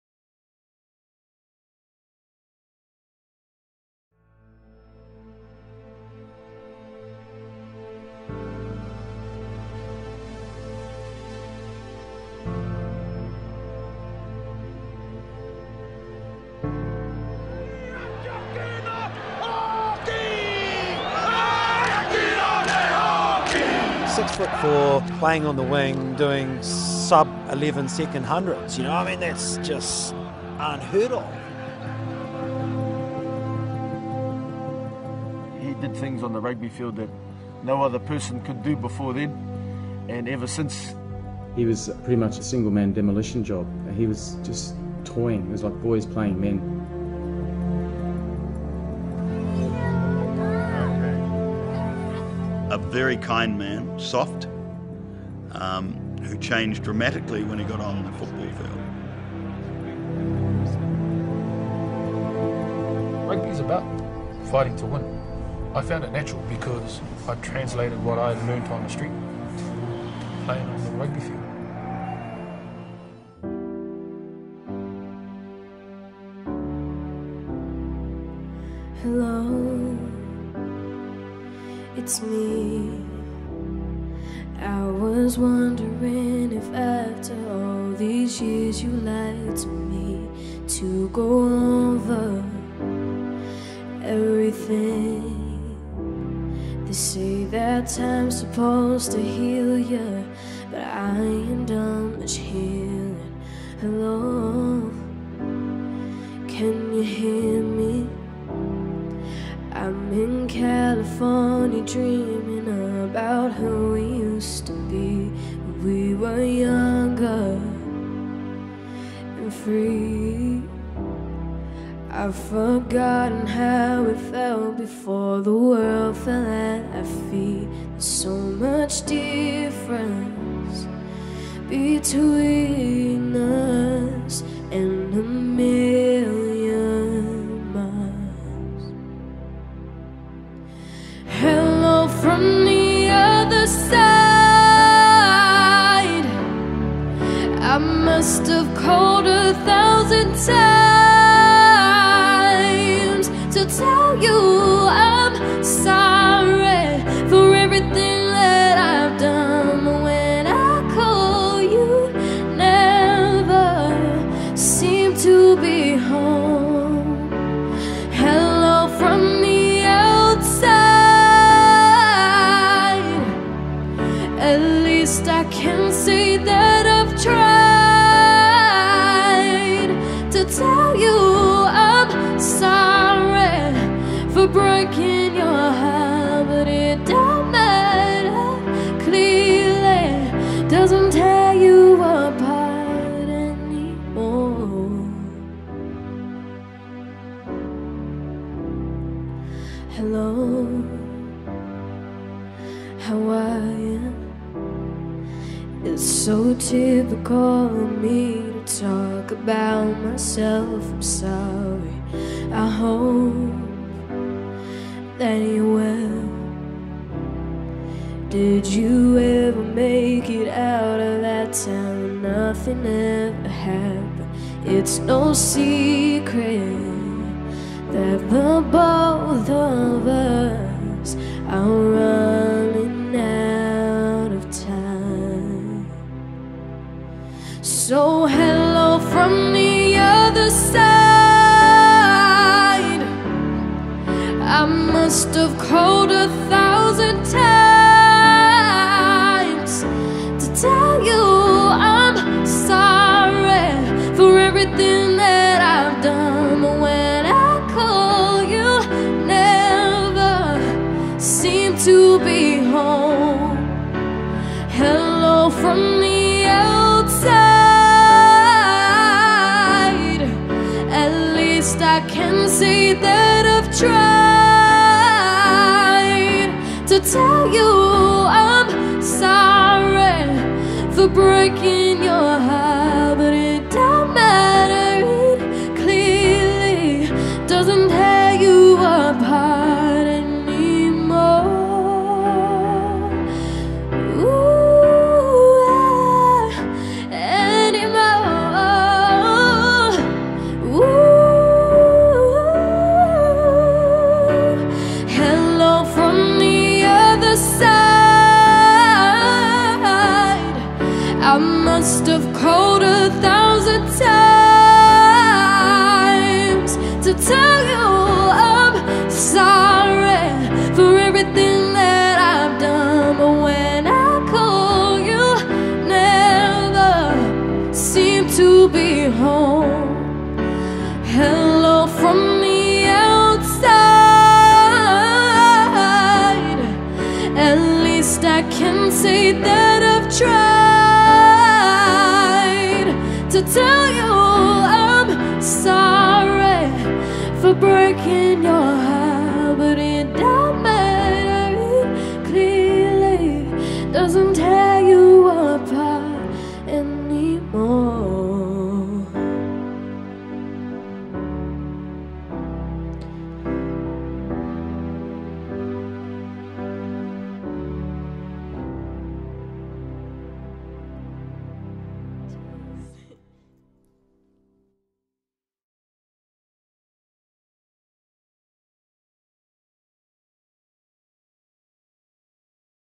JONAH LOMU - Tribute - WORLD RUGBY
He was the first super star of World Rugby. It didn't matter if you had never even seen a game before, you knew who he was. Thank you for making this world just that much better.

Eternal rest, grant unto him, o lord,
And let perpetual light shine upon him.
May he rest in peace.
Amen